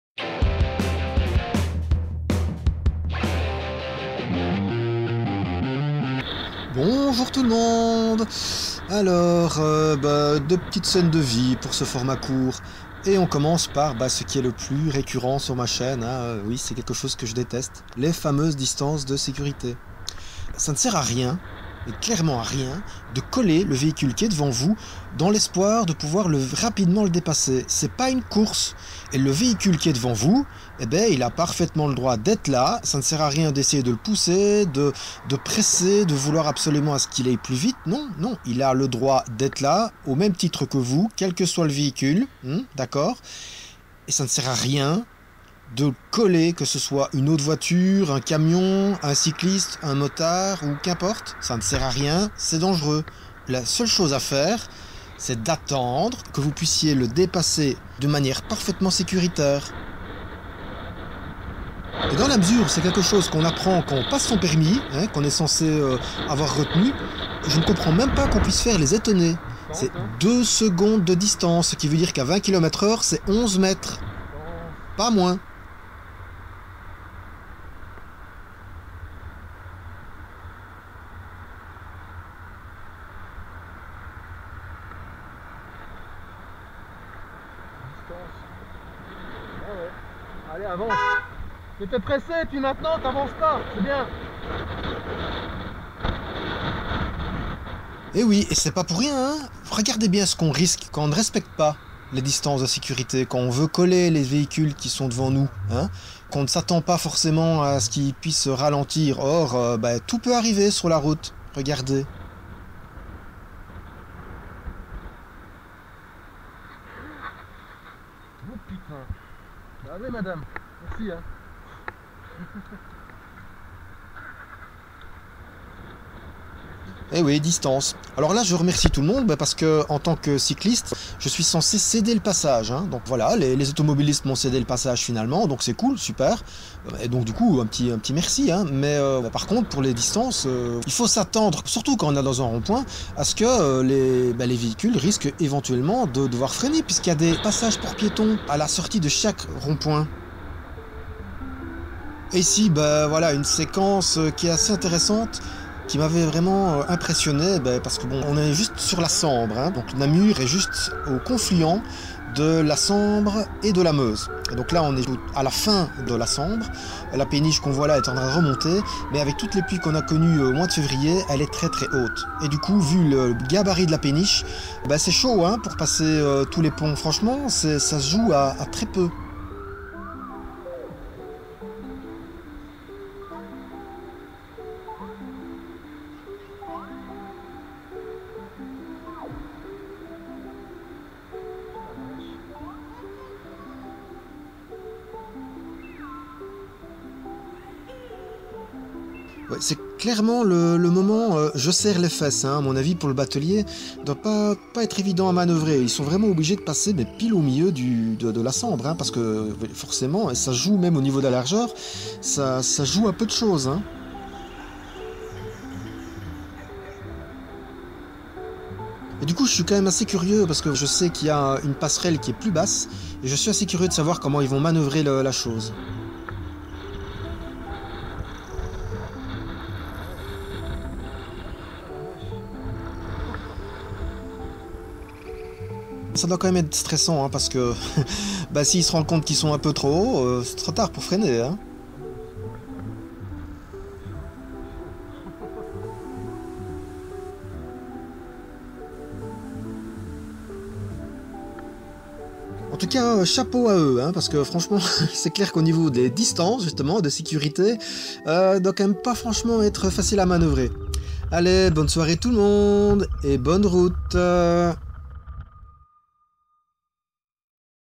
Ce cycliste est obsédé par les distances de sécurité !
--- Filmé le 29 février 2020 ---

Désolé !
Oui, car manque de temps actuellement, il m'a été impossible de finaliser une des vidéos longues qui sont toujours en construction.
Je sors donc un format court ce vendredi.

Quelques petites séquences ... j'en dis pas plus, je vous laisse la surprise.

Vélo : Azub Six
Caméra : Eken H9
Caméra : Mr Handsfree
logiciel de montage : AVS vidéo éditor

Musiques : Proposées pas Youtube, merci. :)